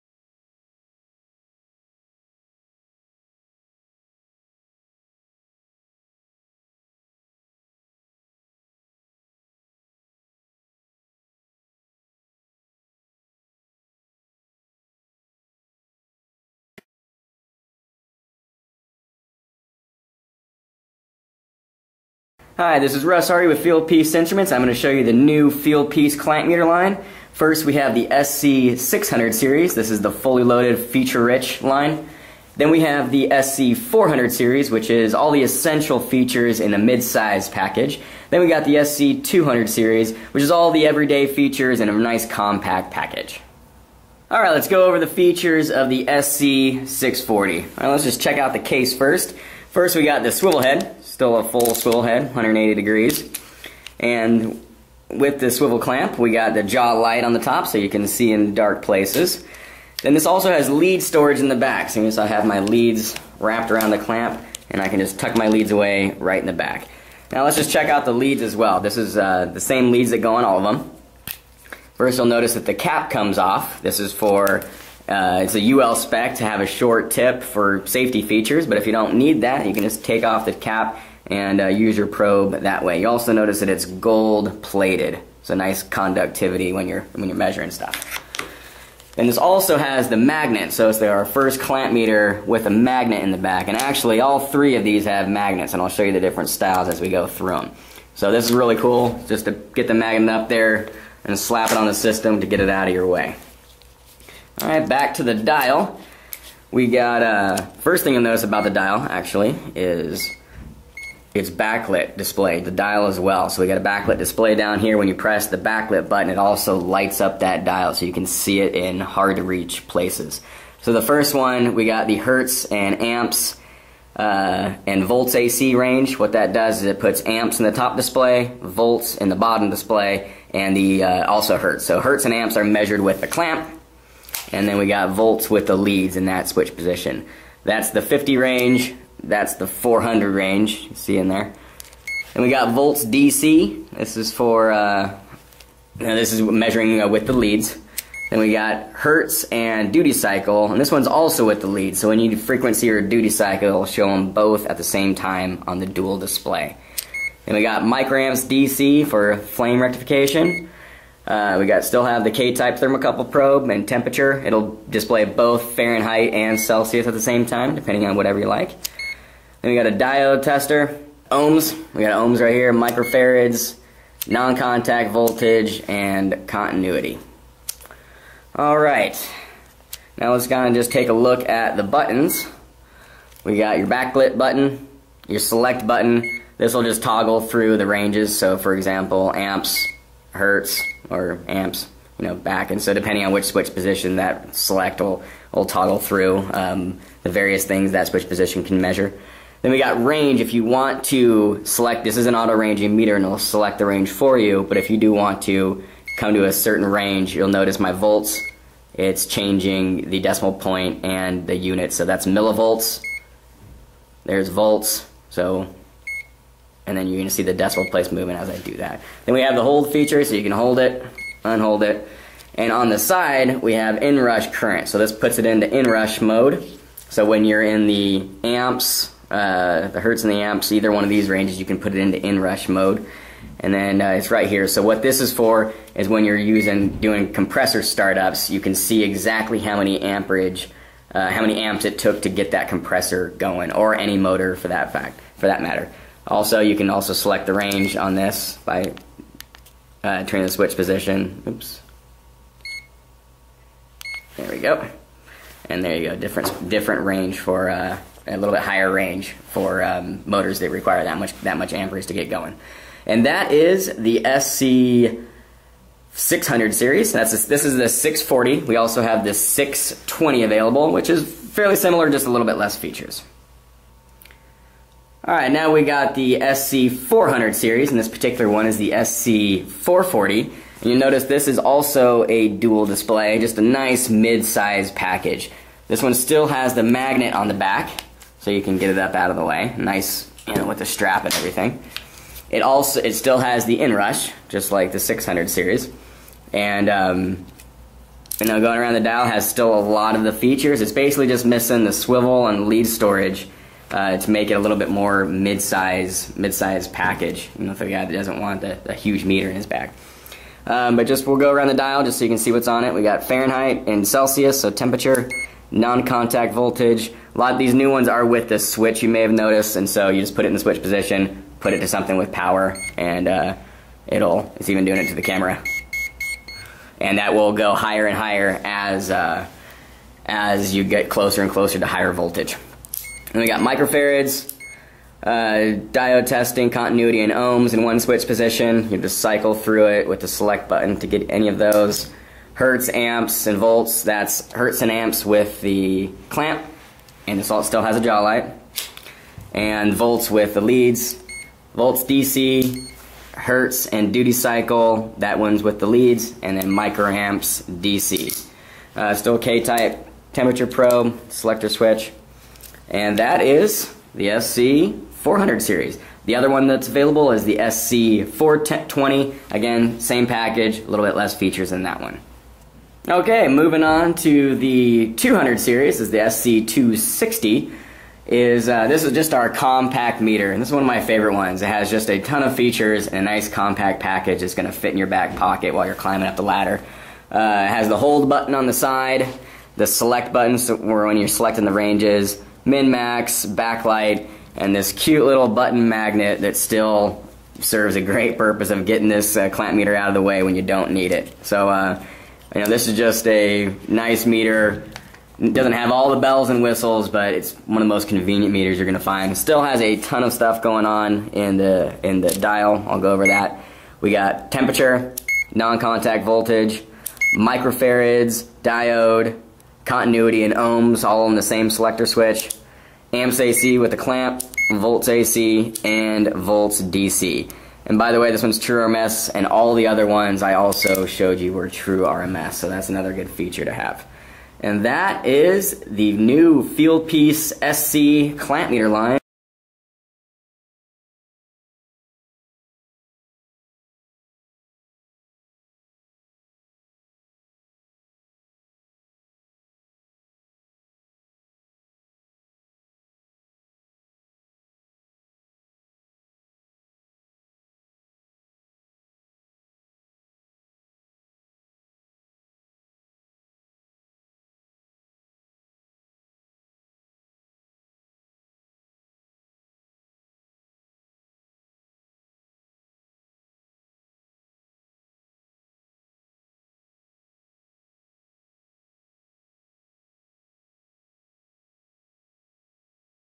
Fieldpiece SC420 Essential Clamp Meter Digital Multimeter
Fieldpiece SC420 is a manual and auto ranging clamp meter for HVACR professionals.